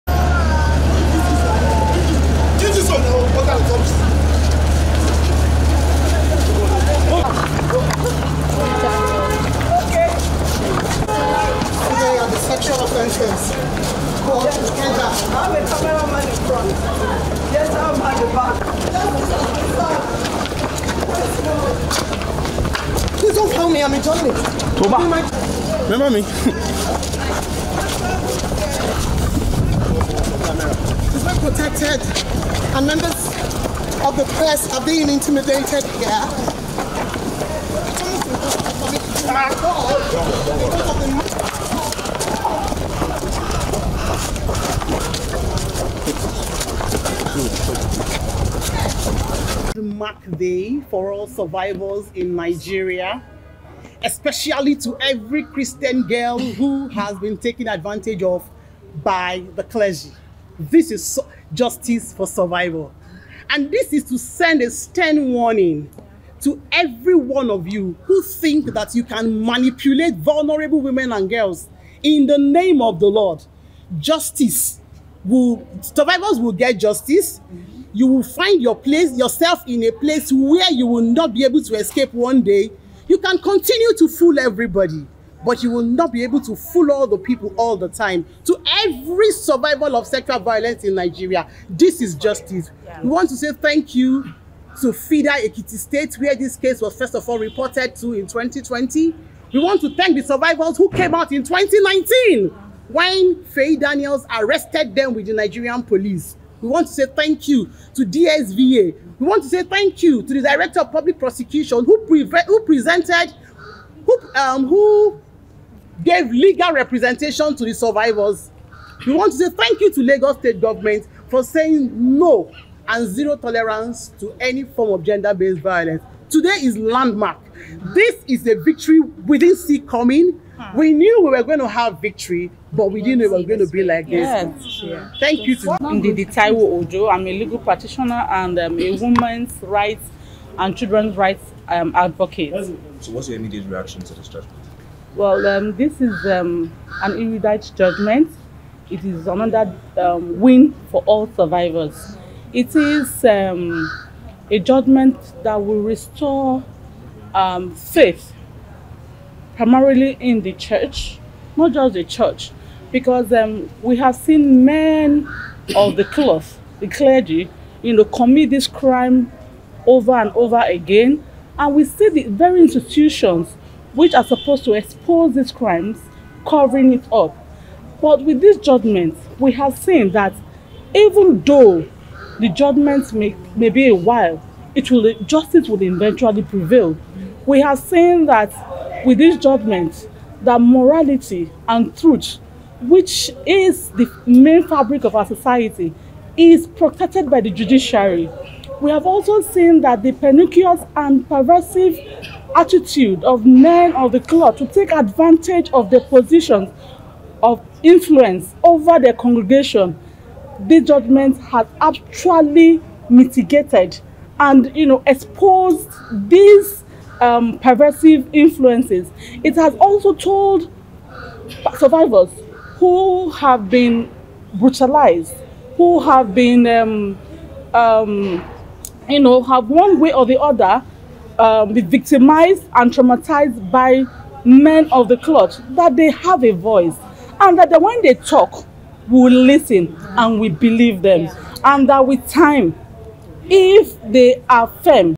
Rapist: Lagos Pastor, Feyi Dainiels, Bags life imprisonment over Sexual Harassment
Mohbad's Death: Lagos CP Revealed That Mohbad Died in Act of Negligence From Quack Auxillary Nurse